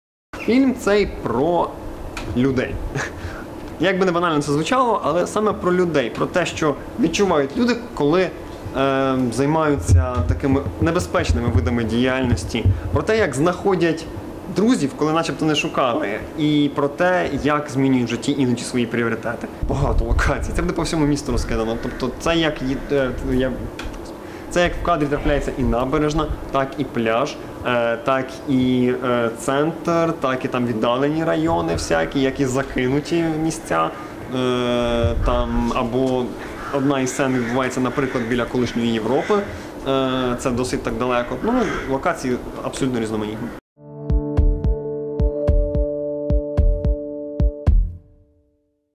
Повнометражний фільм зніматимуть у Хмельницькому
Повнометражне кіно зніматимуть у обласному центрі. Хмельницька студія «Bad Rabbits Foundation» оголосила про початок зйомок повнометражного фільму «Між крадіїв». Жанр майбутньої стрічки – екшн. Це перший повнометражний фільм, що буде знятий суто у Хмельницькому. Які локації задіють та хто виконуватиме ролі дізнавався  Андрій Гуменний.